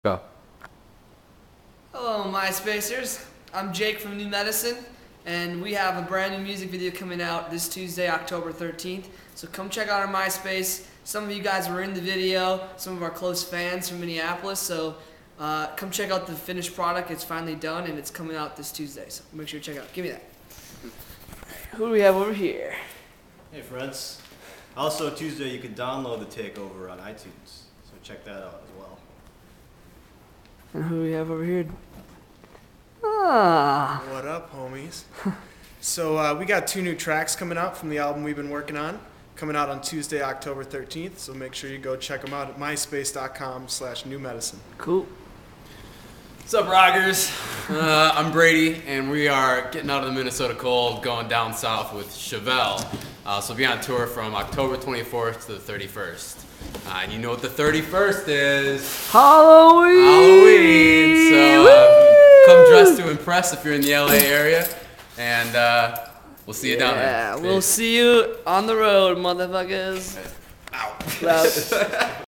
October Update!
New Medicine update about new tour dates this fall as well as music video release!